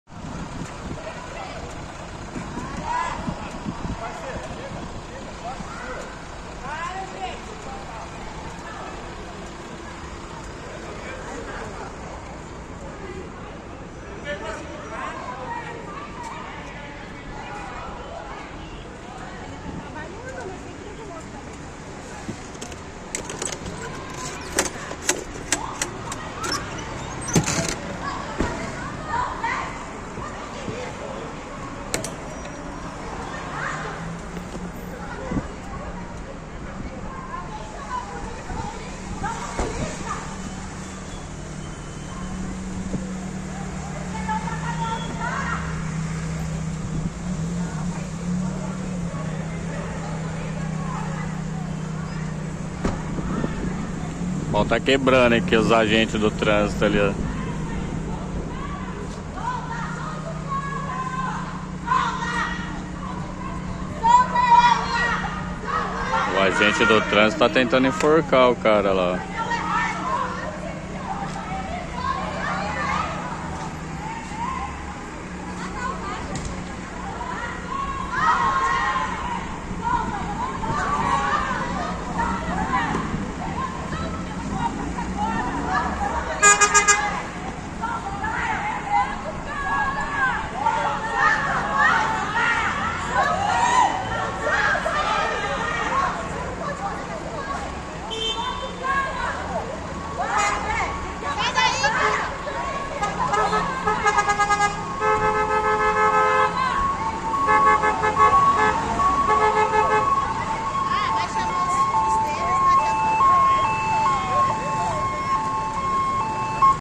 Motociclista agride agente de trânsito no centro de Maringá PR
Motociclista teria furado um bloqueio e na abordagem dos agentes teve inicio a confusão.
Cruzamento das avenidas
Herval com Horácio Racanello